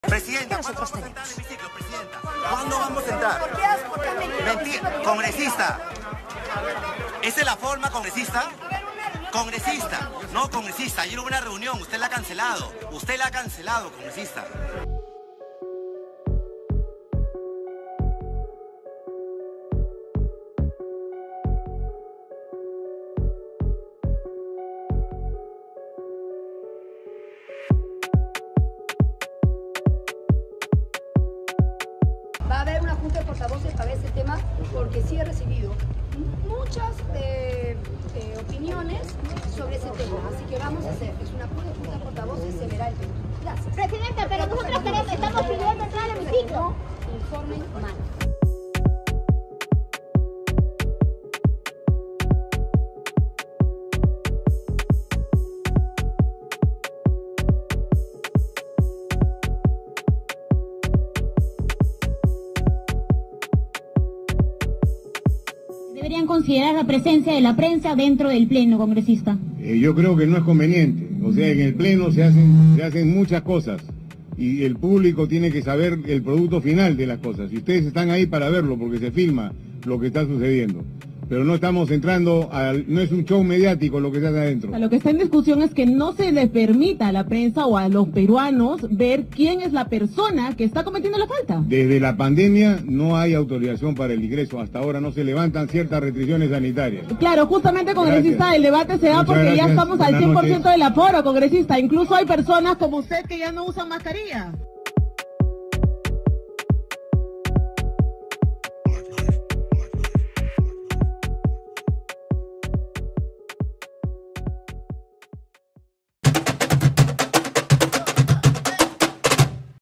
Presidenta del Congreso se altera con periodistas que piden ingresar al Hemiciclo
La presidenta del Congreso volvió a protagonizar un incidente. Esta vez se alteró con la prensa que pedía ingresar al Hemiciclo y otros espacios del Parlamento.